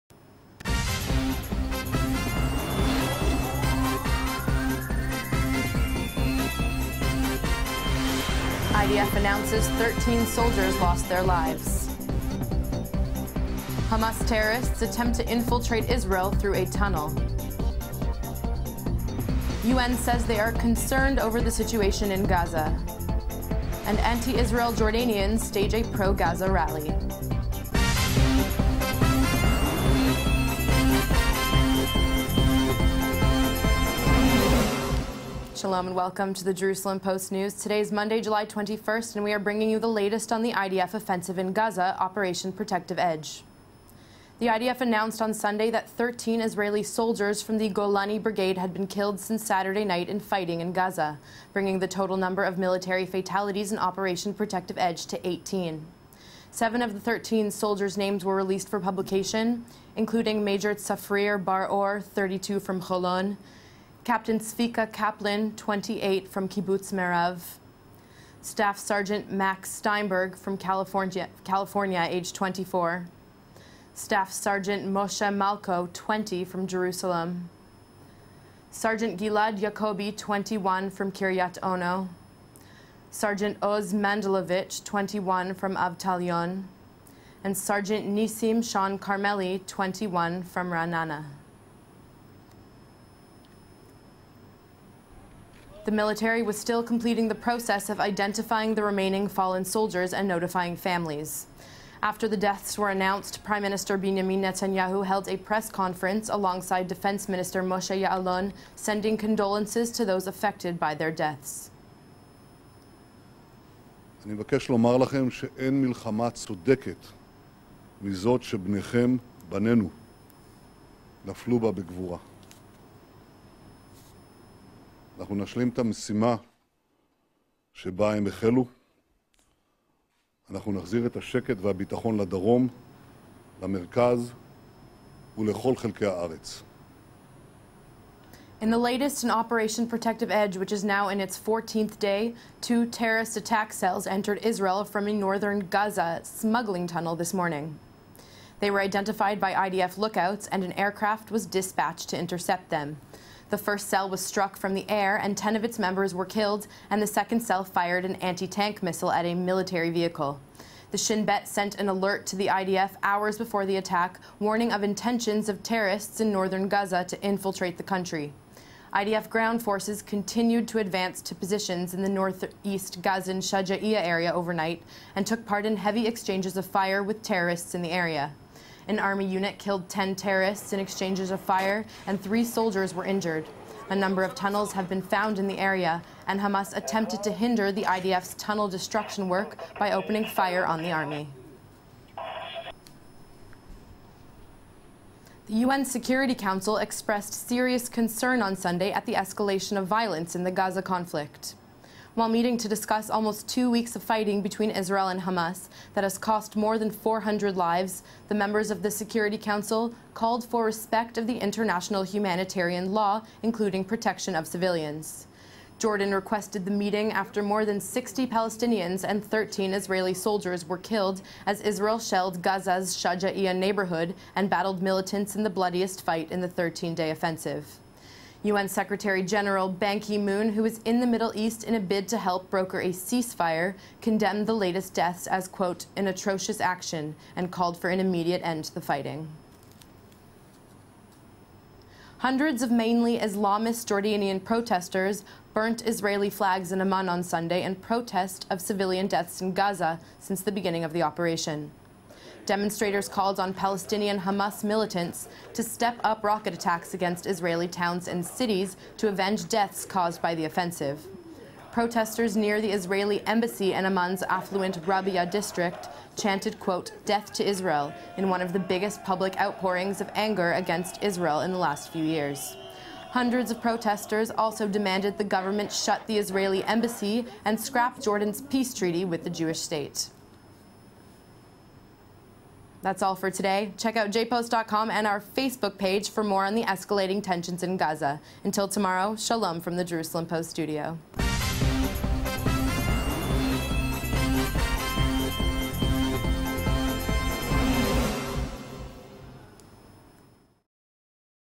JPOST Israel News 21-07-14
The IDF announced on Sunday that 13 Israeli soldiers from the Golani Brigade had been killed since Saturday night in fighting in Gaza, bringing the total number of military fatalities in Operation Protective Edge to 18.

In the latest in Operation Protective Edge, which is now in its 14th day, two terrorist attack cells entered Israel from northern Gaza via a smuggling tunnel this morning.
They were identified by IDF lookouts, and an aircraft was dispatched to intercept them. The first cell was struck from the air, and ten of its members were killed, and the second cell fired an anti-tank missile at a military vehicle. The Shin Bet sent an alert to the IDF hours before the attack, warning of intentions of terrorists in northern Gaza to infiltrate the country.
IDF Ground Forces continued to advance to positions in the northeast Gazan Shejaia area overnight, and took part in heavy exchanges of fire with terrorists in the area. An army unit killed 10 terrorists in exchanges of fire, and three soldiers were injured. A number of tunnels have been found in the area, and Hamas attempted to hinder the IDF's tunnel destruction work by opening fire on the army.
The UN Security Council expressed serious concern on Sunday at the escalation of violence in the Gaza conflict.
While meeting to discuss almost two weeks of fighting between Israel and Hamas, that has cost more than 400 lives, the members of the Security Council called for respect of the international humanitarian law including protection of civilians.
Jordan requested the meeting after more than 60 Palestinians and 13 Israeli soldiers were killed as Israel shelled Gaza's Shejaia neighborhood and battled militants in the bloodiest fighting in the 13-day offensive.
U.N. Secretary-General Ban Ki-moon, who is in the Middle East in a bid to help broker a ceasefire, condemned the latest deaths as quote -an "atrocious action" and called for an immediate end to the fighting.

Hundreds of mainly Islamist Jordanian protesters burnt Israeli flags in Amman on Sunday in protest of the of civilian deaths in Gaza since the beginning of the operation.
Demonstrators called on Palestinian Hamas militants to step up rocket attacks against Israeli towns and cities to avenge deaths caused by the offensive.
Protesters near the Israeli embassy in Amman's affluent Rabia district chanted quote - "Death to Israel" in one of the biggest public outpourings of anger against Israel in the last few years.
Hundreds of protesters, also demanded the government shut the Israeli embassy and scrap Jordan's peace treaty with the Jewish state. More from The Jerusalem Post